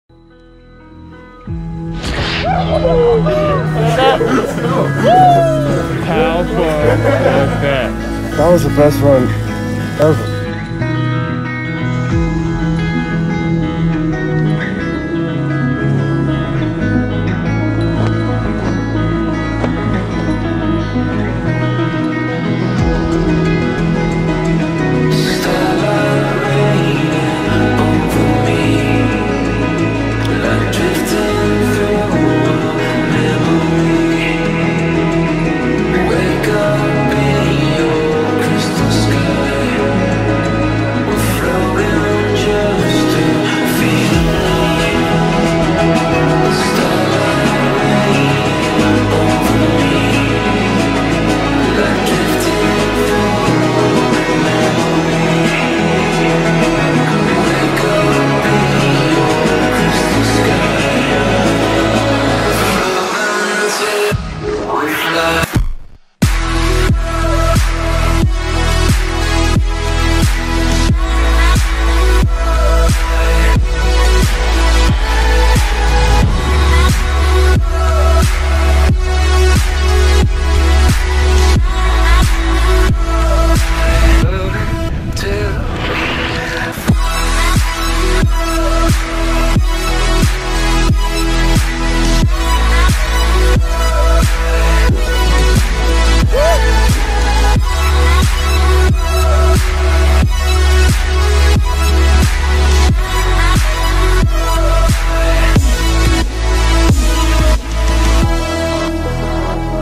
Canada Powder Tour - Living the Dream!
Want to know what it's like joining a Mountainwatch Canada Powder Tour? This video shows it all! It's not just the amazing scenery and deep, DEEP pow! It's nothing but good times, experiencing some of the best Canadian ski towns and riding as much pow as possible!

Sounds like you? Check the link below how you can live the dream!